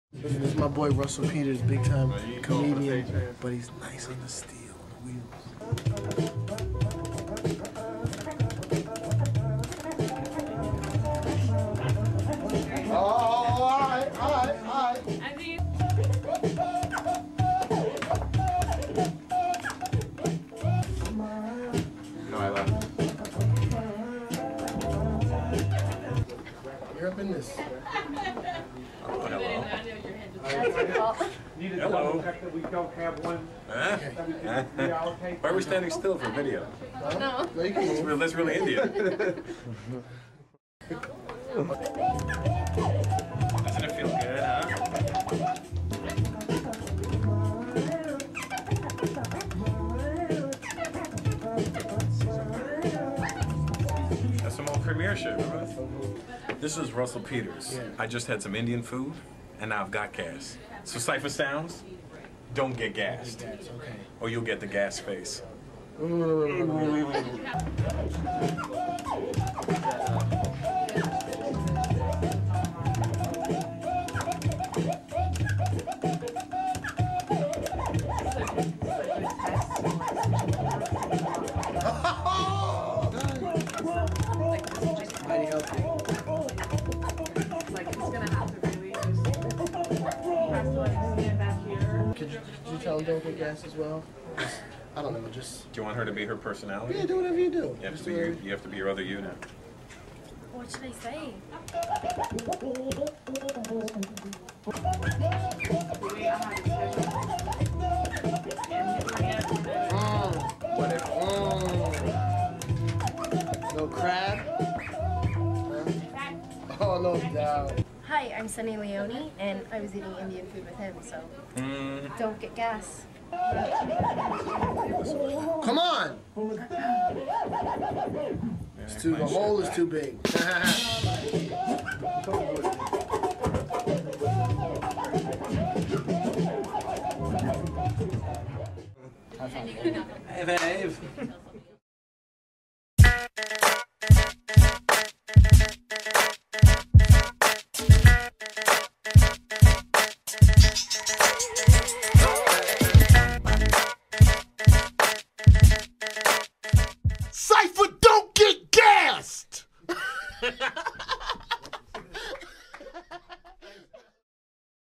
COMEDIAN RUSSELL PETERS AND CIPHA SOUNDS IN A DJ BATTLE
COMEDIAN RUSSELL PETERS BATTLES CIPHA SOUNDS IN A DJ BATTLE...